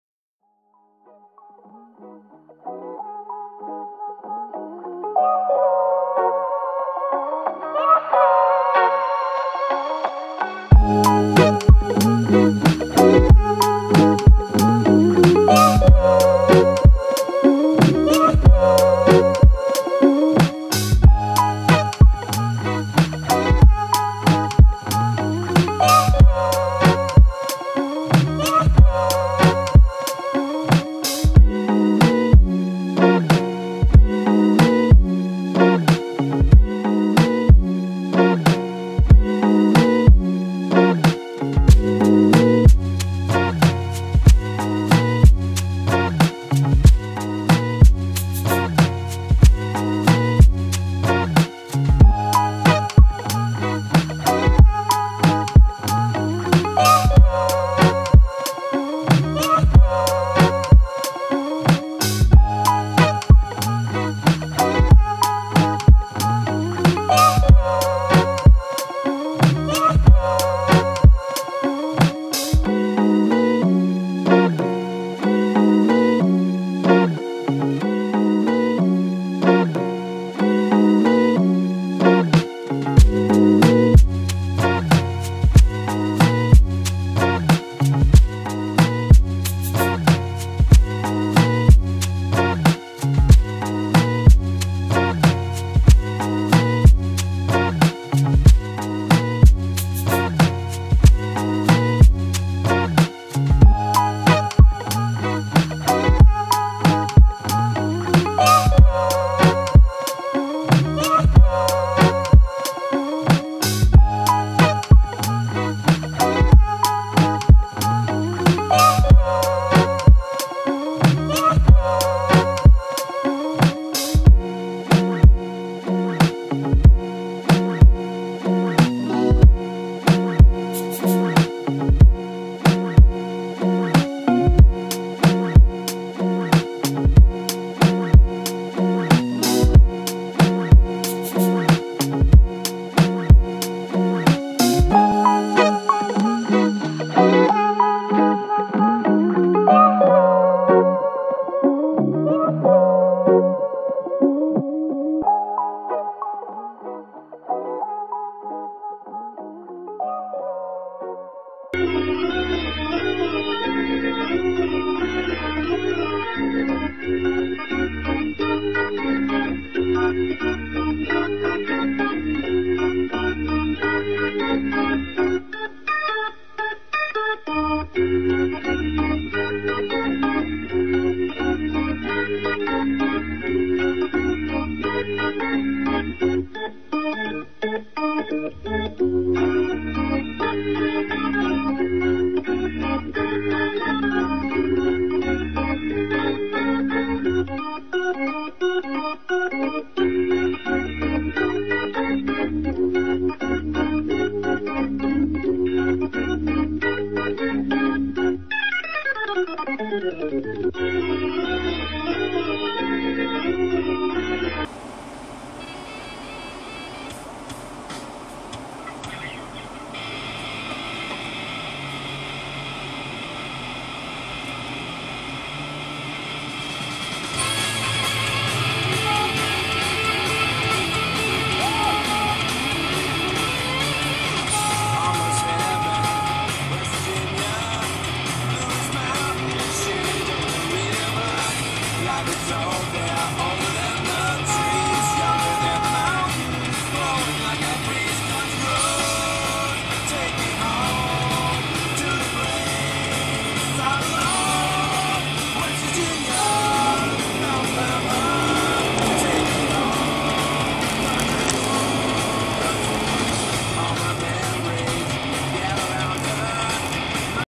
Dashcam - Random Freeway Stoppage
I have no idea why they decided to stop traffic on the freeway.  That middle section is sped up 10x.

As far as the Toyota?  Couldn't see them move a muscle at any point.  As far as I know it's still there.

I need a louder horn.